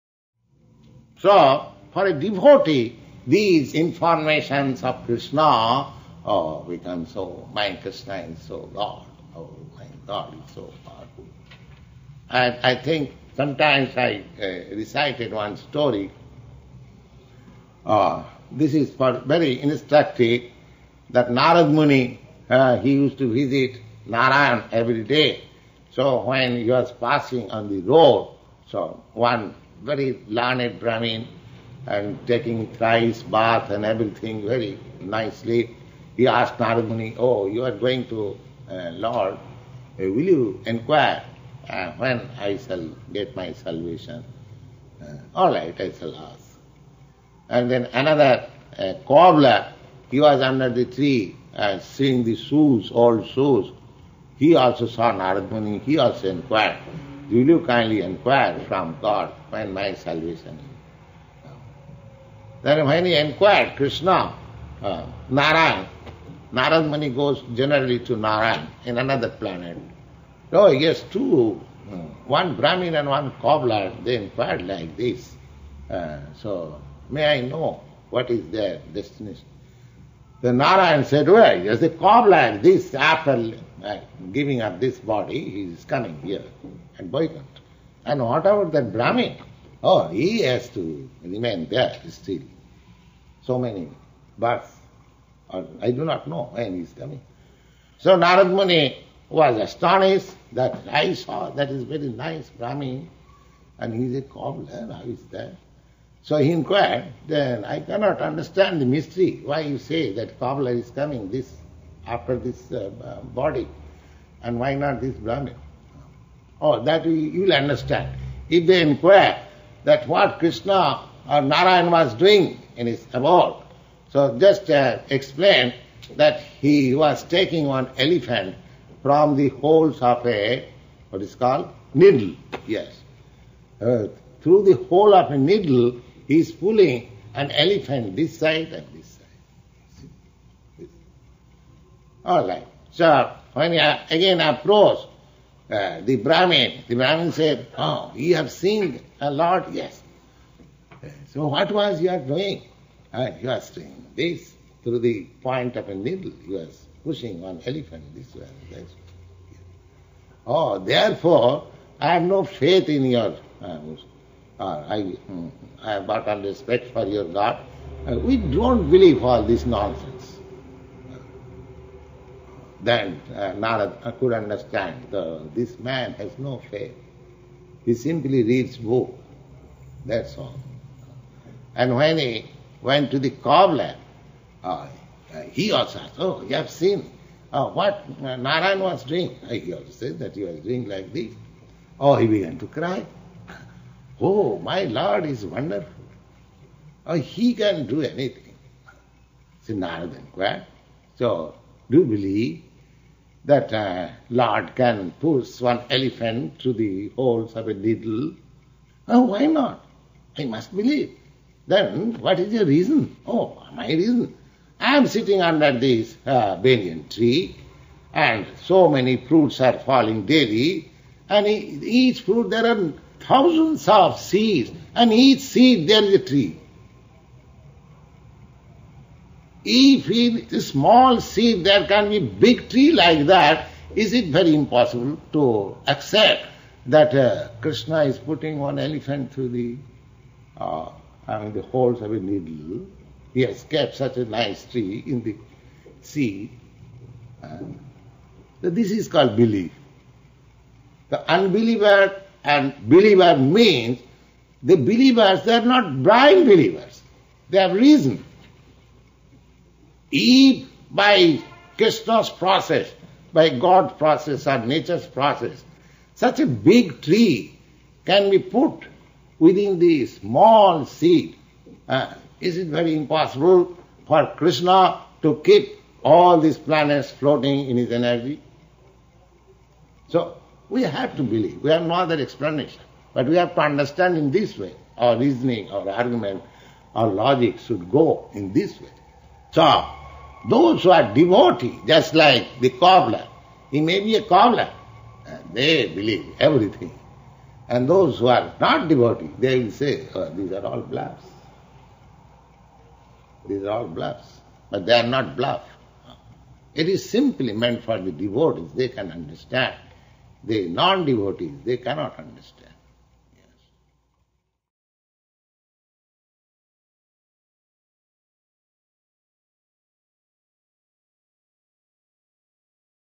Story: The Brahmana and the Cobbler
[The Story of The Cobbler and The Brahmana]

In this regard, there is a story of Narada Muni, who was once asked by a brahmana: Oh, you are going to meet the Lord? Will you please ask Him when Im going to get my salvation?

All right, Narada agreed. I shall ask Him.

As Narada proceeded, he met a cobbler who was sitting under a tree mending shoes, and the cobbler similarly asked Narada, Oh, you are going to see God? Will you please inquire of Him when my salvation will come?

When Narada Muni went to the Vaikuntha planets, he fulfilled their request and asked Narayana (God) about the salvation of the brahmana and the cobbler, and Narayana replied, After leaving this body, the cobbler shall come here to me.

What about the brahmana? Narada asked.

He will have to remain there for a number of births. I do not know when he is coming.
Narada Muni was astonished, and he finally said, I cant understand the mystery of this.

That you will see, Narayana said. When they ask you what I am doing in My abode, tell them that I am threading the eye of a needle with an elephant.

When Narada returned to earth and approached the brahmana, the brahmana said, Oh, you have seen the Lord? What was He doing?

He was threading an elephant through the eye of a needle, Narada answered.

I dont believe such nonsense, the brahmana replied. Narada could immediately understand that the man had no faith and that he was simply a reader of books.

Narada then left and went on to the cobbler, who asked him, Oh, you have seen the Lord? Tell me, what was He doing?

He was threading an elephant through the eye of a needle, Narada replied.

The cobbler began to weep, Oh, my Lord is so wonderful, He can do anything.

Do you really believe that the Lord can push an elephant through the hole of a needle? Narada asked.

Why not? the cobbler said, Of course I believe it.

How is that?

You can see that I am sitting under this banyan tree, the cobbler answered, and you can see that so many fruits are falling daily, and in each seed there is a banyan tree like this one. If, within a small seed there can be a big tree like this, is it difficult to accept that the Lord is pushing an elephant through the eye of a needle?

So this is called faith. It is not a question of blindly believing. There is reason behind the belief. If Krsna can put a large tree within so many little seeds, is it so astounding that He is keeping all the planetary systems floating in space through His energy?

Although scientists may think that the planets are being held in space simply by nature alone, behind nature there is the Supreme Lord. Nature is acting under His guidance. As Sri Krsna states:

mayadhyaksena prakrtih
suyate sa-caracaram
hetunanena kaunteya
jagad viparivartate

This material nature is working under My direction, O son of Kunti, and is producing all moving and unmoving beings. By its rule this manifestation is being created and annihilated again and again. (Bg. 9.10)
Mayadhyaksena means under My supervision. Material nature cannot act so wonderfully unless the Lords hand is behind it. We cannot give any example of material things automatically working. Matter is inert, and without the spiritual touch there is no possibility of its acting. Matter cannot act independently or automatically. Machines may be very wonderfully constructed, but unless a man touches that machine, it cannot work. And what is that man? He is a spiritual spark. Without spiritual touch, nothing can move; therefore everything is resting on Krsnas impersonal energy. Krsnas energy is impersonal, but He is a person. We often hear of persons performing wonderful actions, yet despite their energetic accomplishments, they still remain persons. If this is possible for human beings, why isnt it possible for the Supreme Lord? We are all persons, but we are all dependent upon Krsna, the Supreme Person.